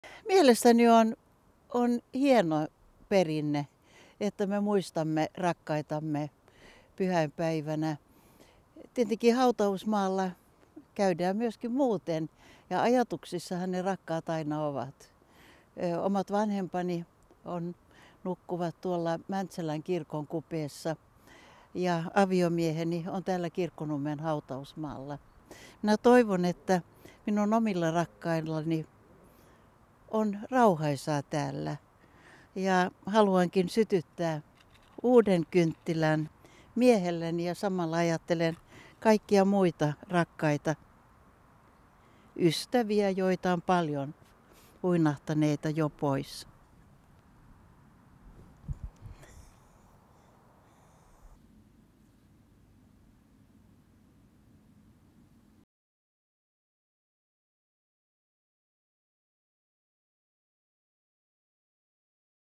Pyhäinpäivä: Elisabeth Rehn
Ministeri Elisabeth Rehn sytyttää kynttilän Ove-puolisonsa muistoksi.